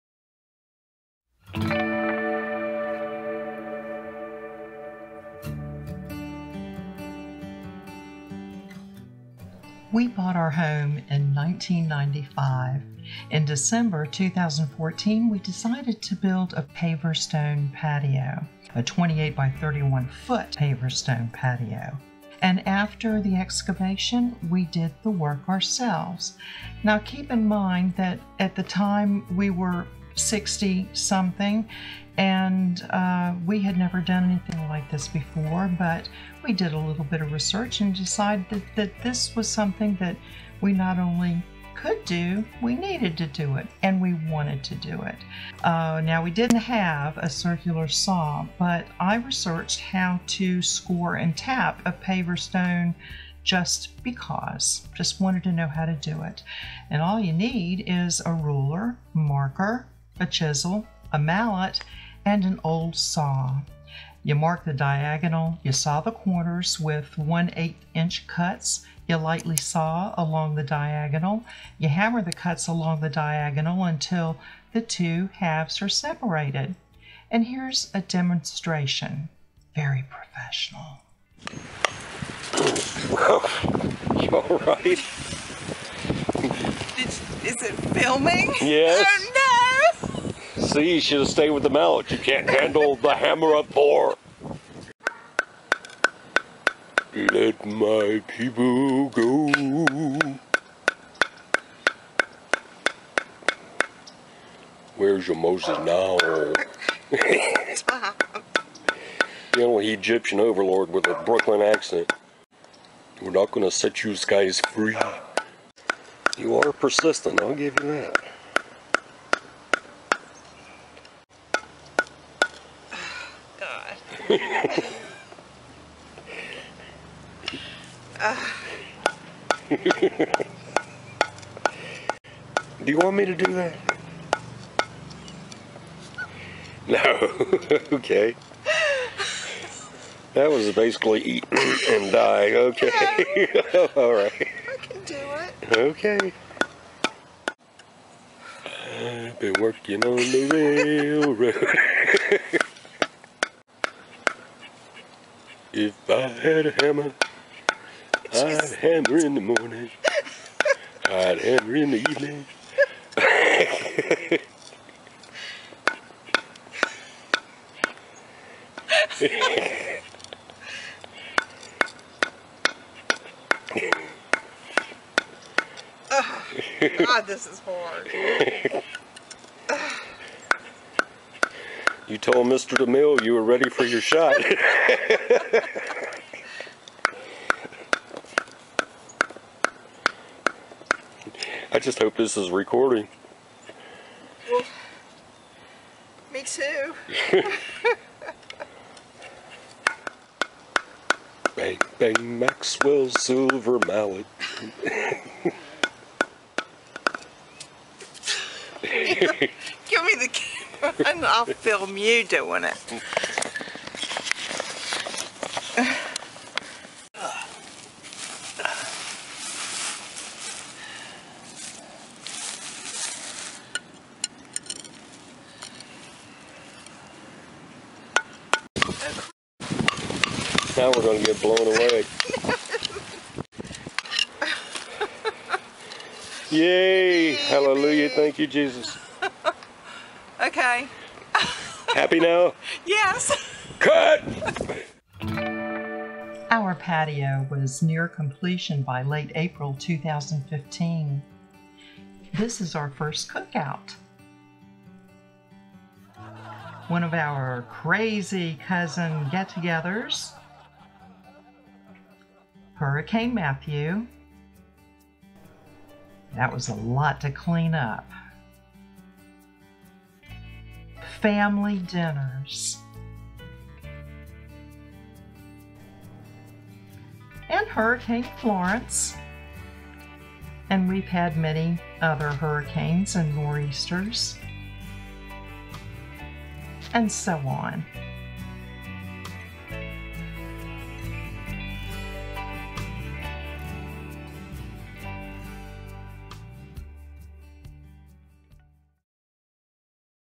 2014 PatioProject
Old(er) couple decides to build 28' x 31' paver stone patio.